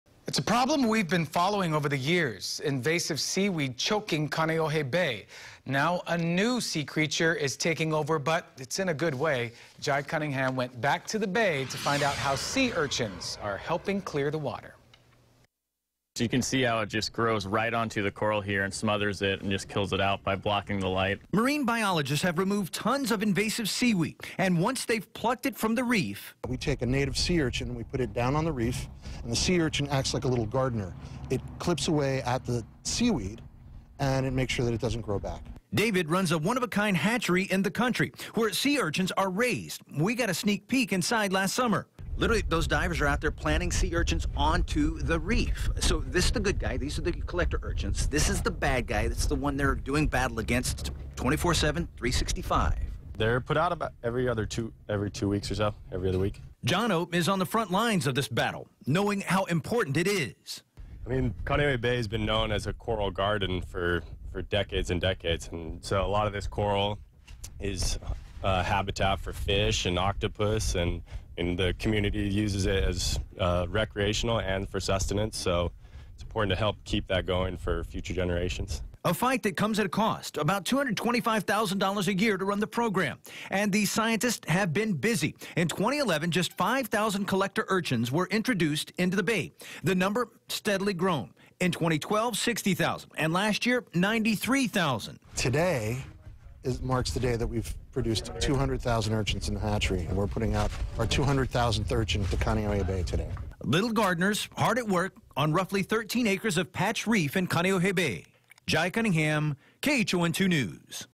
Native sea urchins take back Kaneohe Bay
For years, invasive seaweed has plagued Kaneohe Bay, but marine biologists say a new sea urchin is working to clear the water.